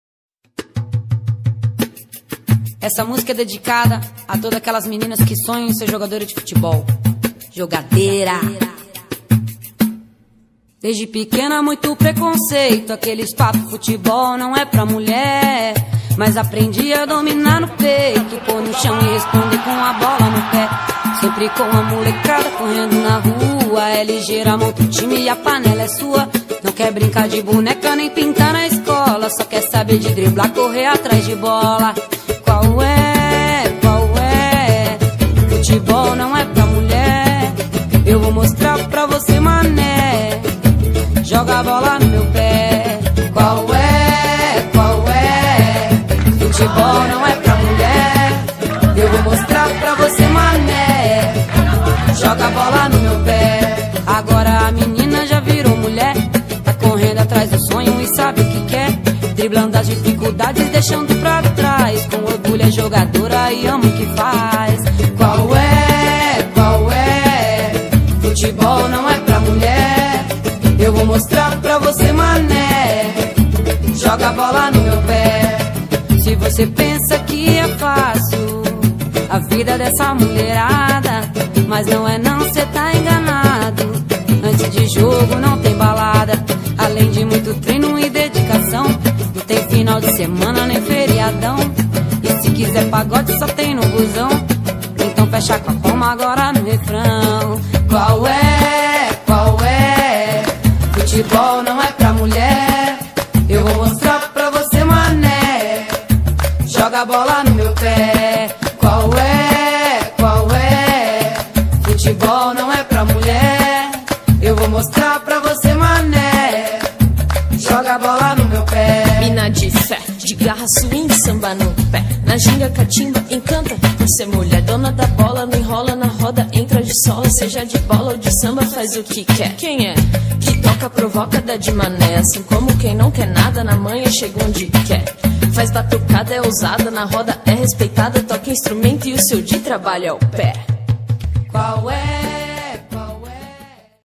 Melhores fotos de Cristiane Rozeira (Mamãe Cris)❤
Melhores fotos da mamãe Cris, artilheira, jogadeira, uma das melhoresss, uma inspiração para todas as meninas que sonham em ser jogadora de futebol ❤❤